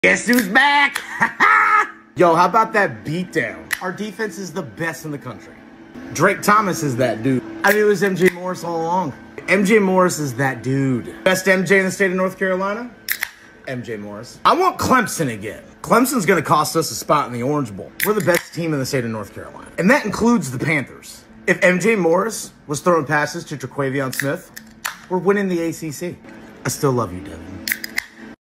NC State fans after beating Wake Forest
How NC State fans are feeling after the win over Wake Forest